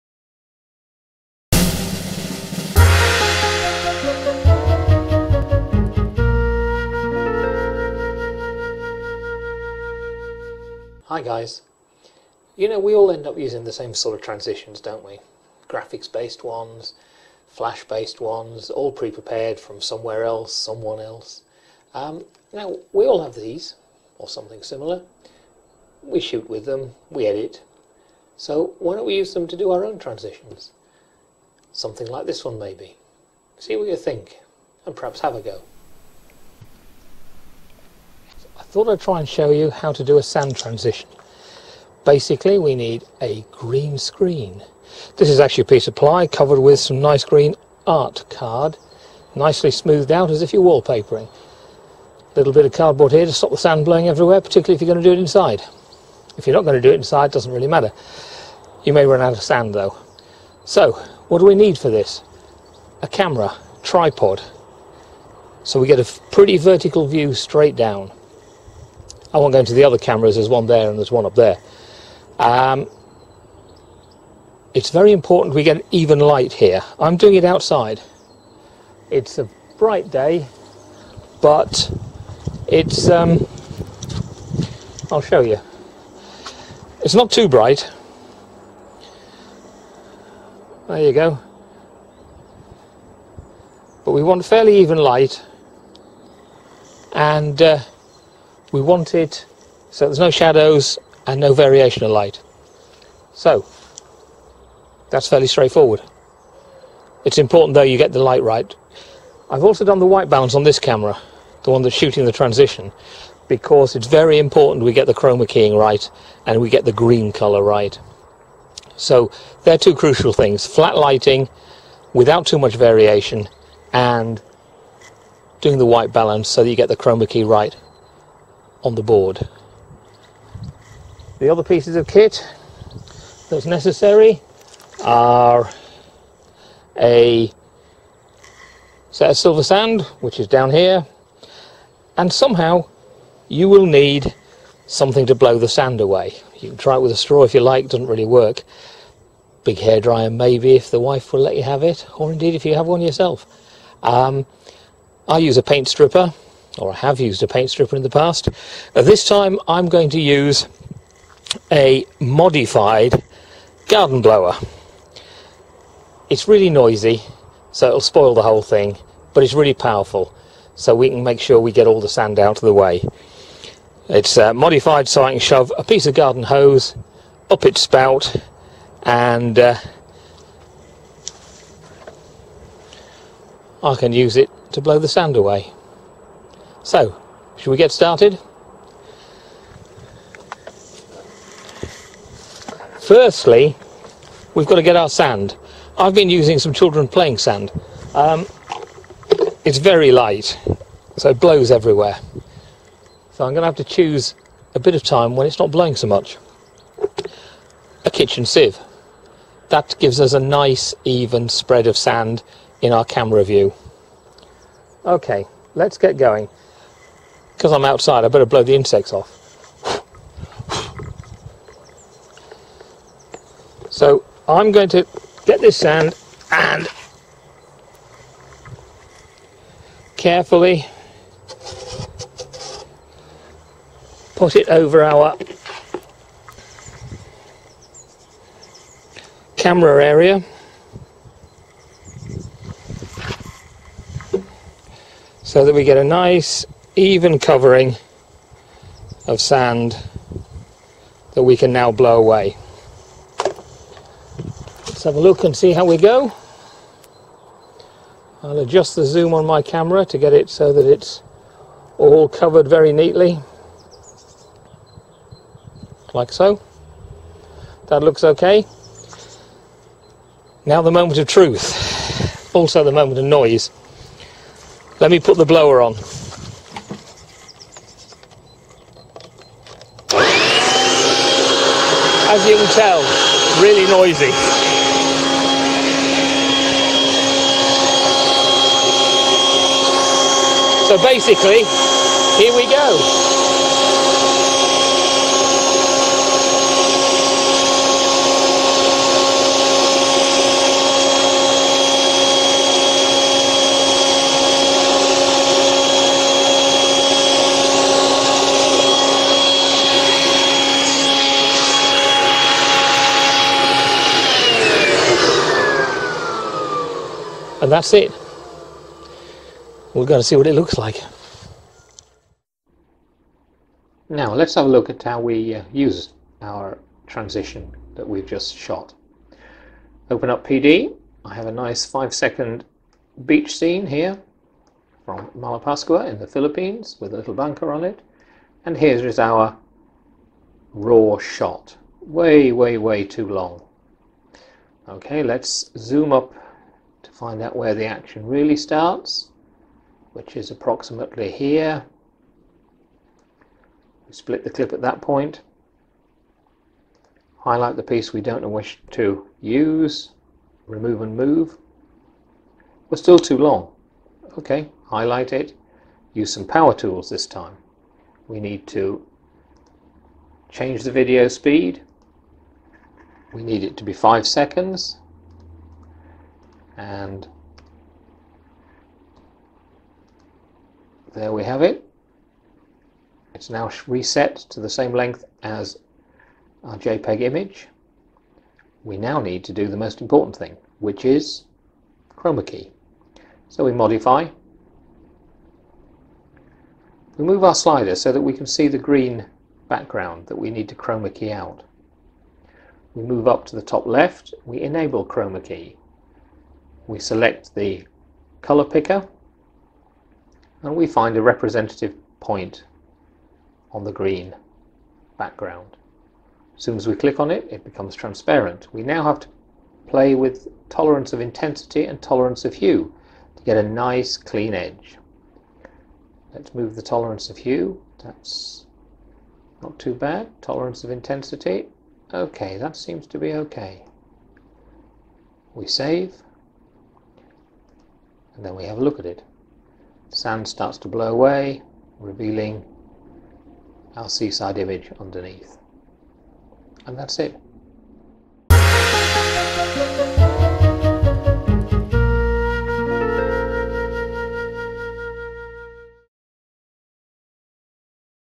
Creating A Custom Blowing Sand Transition - PowerDirector Tutorial
How to film a custom transition, and apply it to footage using the chroma key tool in PowerDirector11.